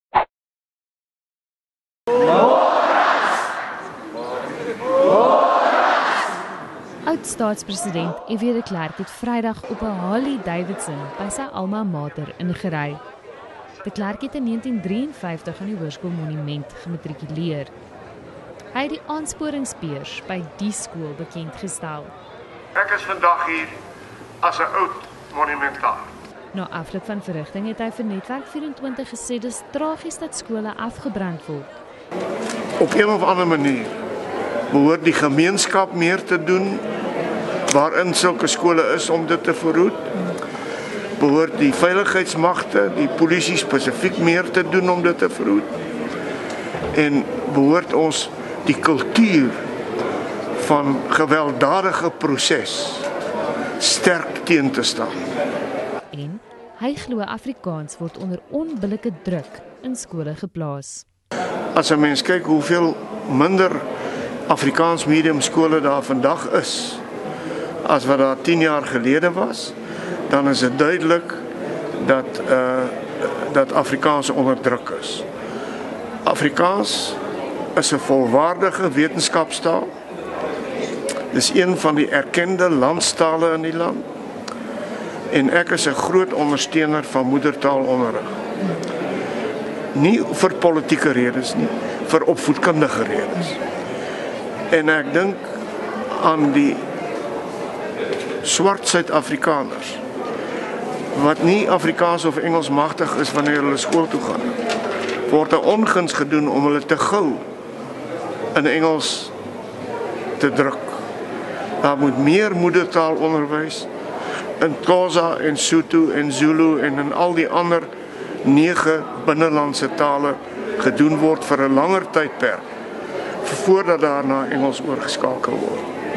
F.W. ry draai op Harley by Monnas
“Dit is duidelik dat Afrikaans onderdruk word as ’n mens kyk hoeveel minder Afrikaansmedium skole daar vandag is in vergelyking met tien jaar gelede,” het oudpres. F.W. de Klerk Vrydag gesê.
Hy het die F.W. de Klerk-aansporingsbeurs by die Hoërskool Monument in Krugersdorp bekend gestel. Hy was ’n leerling van die skool.